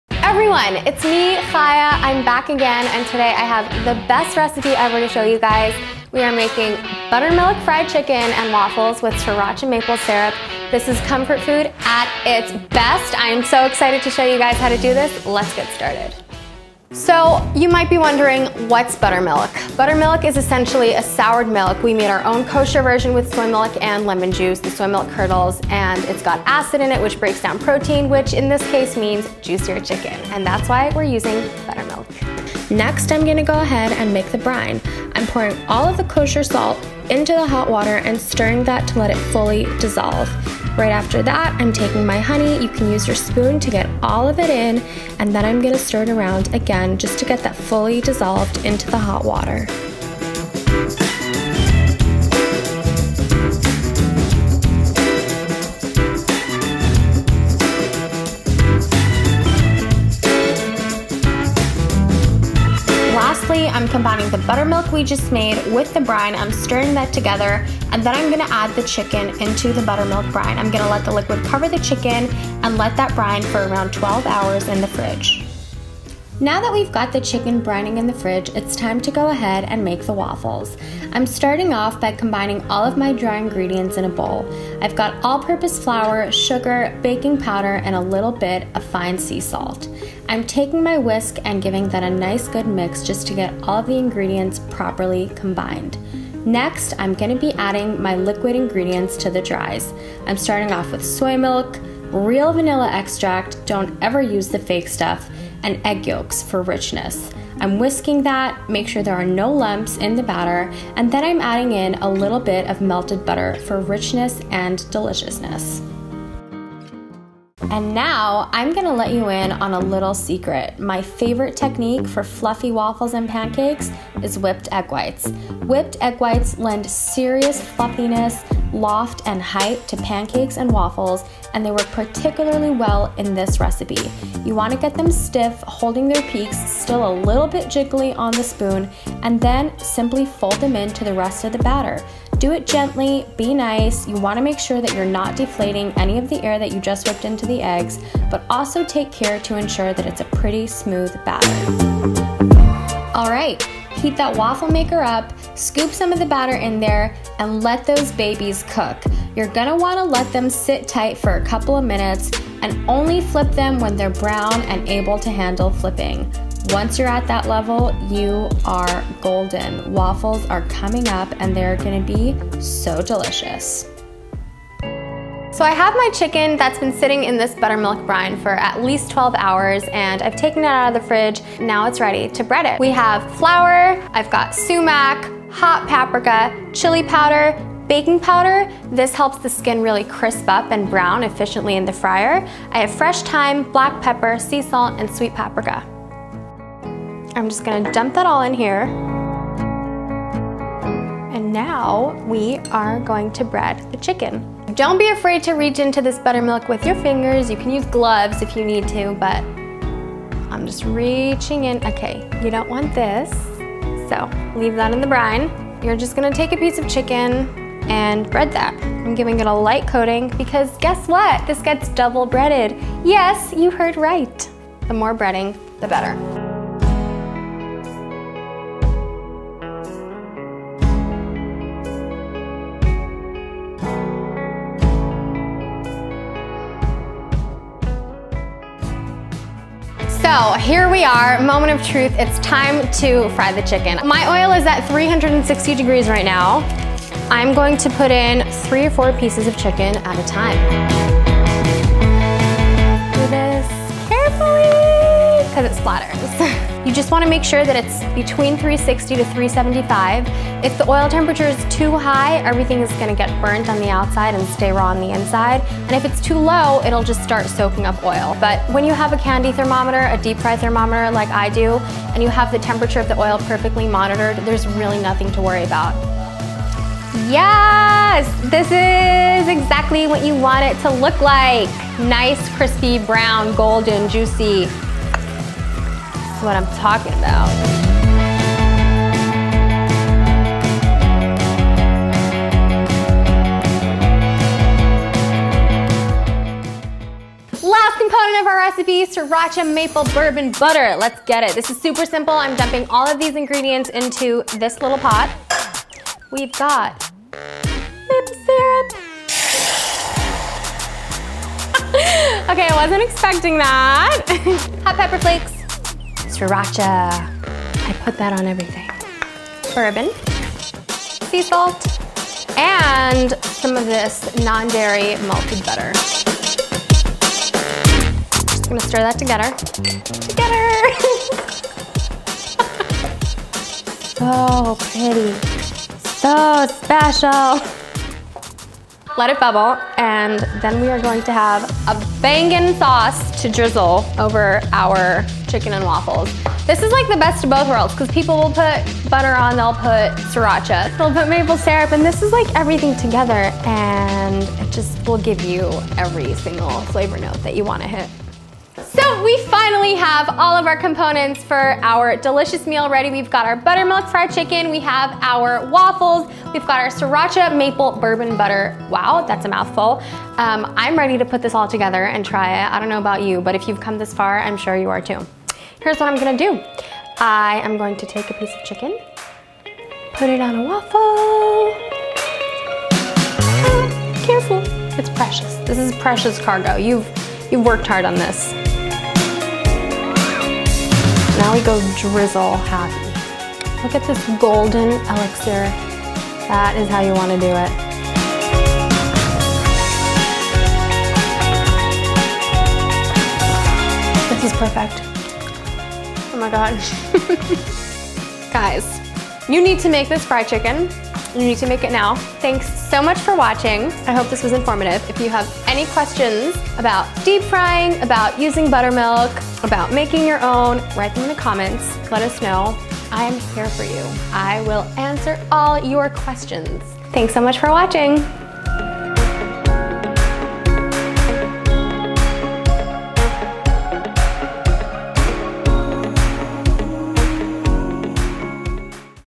Awesome Buttermilk Fried Chicken and Waffles - How To
Fried Chicken and Waffles are a staple of Southern-Style Soul Food. While the exact origin of this unlikely pairing remains subject to debate, its early 20th century prominence amongst New York’s African-American owned eateries cemented it as a staple of American cuisine.

This recipe combines a zingy lemon and thyme chicken, fluffy egg-white waffles, with a sweet and spicy Sriracha-Maple Syrup sauce.

Recipe below!

Connect with Live Kosher!
Follow todays host, Chaya Rappoport!
Connect with Kettle and Cord!

Ingredients:
You will need:
4 cup soy milk
½ lemon (juice)
1 lemon (peeled, seeded & halved) ½ cup honey
2 cup hot water
1/3 cup kosher salt
6 bay leaves
3 cloves garlic (smashed)
6 sprigs thyme
8 boneless, skin on chicken thighs
Breading mixture:
3 cup all-purpose flour
2 tsp baking powder
2 tbsp chili powder
3 sprigs thyme
2 tsp sweet paprika
1 tsp sumac
1 tbsp hot paprika
2 tsp kosher salt
½ tsp black pepper
1 qt canola or vegetable oil (for frying)
Sriracha maple butter:
½ cup maple syrup
2 tbsp sriracha
2 tbsp bourbon
4 tbsp melted butter (Earth Balance) ¼ tsp fine sea salt
¼ tsp red pepper flakes
Waffles:
2 cup flour
½ cup sugar
1 tbsp baking powder
1 cup melted butter
½ tsp sea salt
1½ cup soy milk
2 egg yolks
1 tbsp pure vanilla extract
4 egg whites (whipped)
Directions:
•  In a large food safe container, add the soy milk & juice of ½ a lemon. Let it sit about 10 mins until it curdles. This is the “buttermilk”. To the buttermilk, add the peeled, seeded and halved lemon, bay leaves, smashed garlic & thyme. Dissolve salt & honey in the hot water. This is the brine. Stir the brine into the buttermilk and spices, then add chicken.
•  Refrigerate for 1 day. The next day, combine the flour, baking powder, chili powder, thyme, sumac and sweet and hot paprika, salt & pepper in a large bowl. Mix thoroughly. Remove chicken from buttermilk brine, but do not throw away the brine. Coat the wet chicken in a thin layer of the flour mixture, and bring to room temperature on drying rack set over paper towels.
•  Heat oil to 370° degrees in large cast iron or heavy bottomed pot. When chicken is close to room temperature, dip in reserved buttermilk brine & once into flour mixture then drop gently into the hot oil. Fry 3 pieces at a time. Cook for 4-5 mins on each side, until golden brown. Thermometer should reach 165°. Remove from oil and sprinkle with fresh thyme. Repeat with remaining chicken pieces.
•  To make the waffles: preheat the waffle iron to regular setting. Sift together the flour, sugar, baking powder and salt in a bowl. In a separate bowl, whisk together the milk, vanilla and 2 egg yolks. Pour over the dry ingredients and very gently stir until halfway combined. Pour in the melted butter and continue mixing very gently until combined. In a separate bowl (or using a mixer), beat the 4 egg whites with a whisk until stiff. Slowly fold them into the batter, stopping short of mixing them all the way through. Scoop the batter into your waffle iron in batches and cook according to the manufacturer’s directions. Keep warm. In a small saucepan, combine the ingredients for the spicy maple butter. Let come to a boil.
•  Serve drizzled over the hot chicken, immediately. To serve: Place 2 chicken thighs on top of a waffle. Pour the warm sauce over everything and serve.